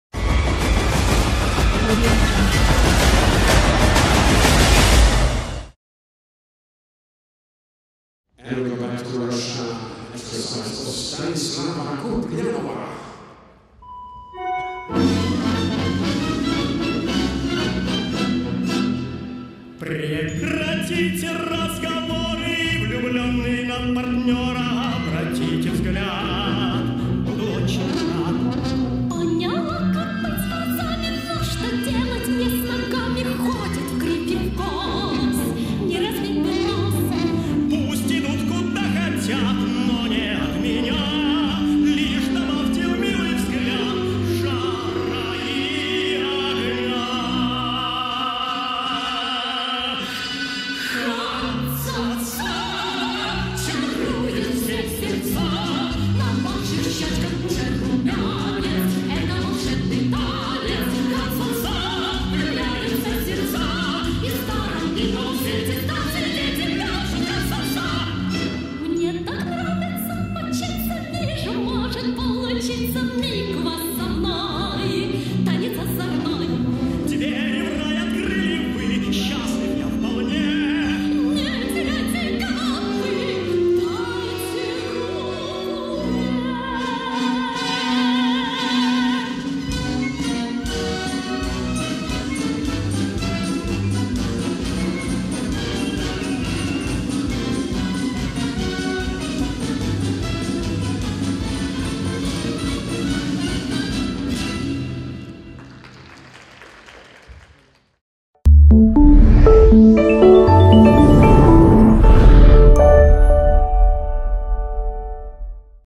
KUPRIIANOVA STANISLAVA 2011 RUS
FIG and children international competition «LUXEMBOURG CUP 2019» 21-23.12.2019, Luxembourg, Belair
KUPRIIANOVA STANISLAVA Ryazan SSHOR AKADEMIA